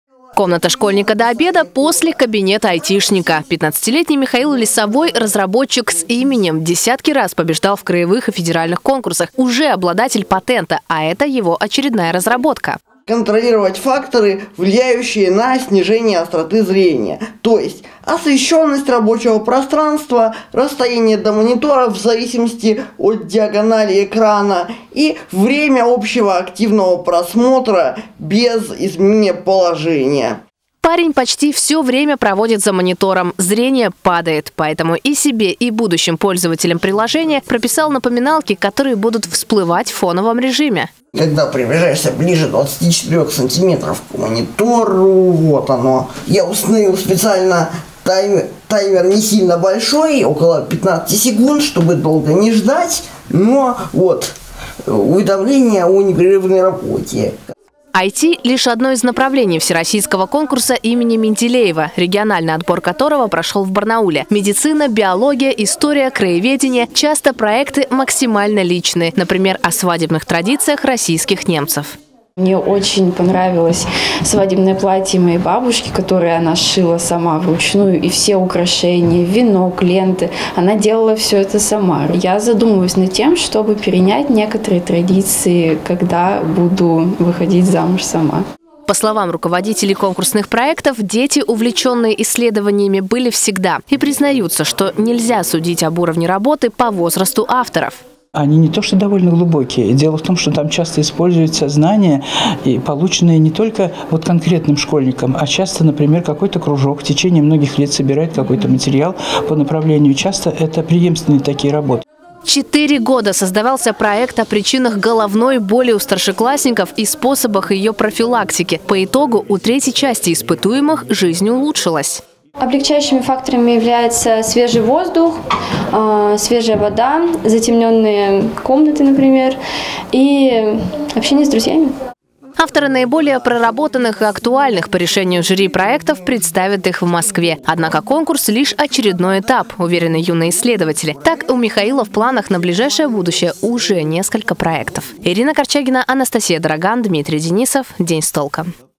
В Барнауле прошел региональный отбор всероссийского конкурса имени Менделеева
Приложение для защиты вашего зрения, краеведческая экспедиция на Рудный Алтай, новые привычки для здоровья старшеклассников. В Барнауле прошел региональный отбор всероссийского конкурса имени Менделеева. В нем свои исследования и разработки защищают – школьники. О чем думают молодые умы и что хотят изменить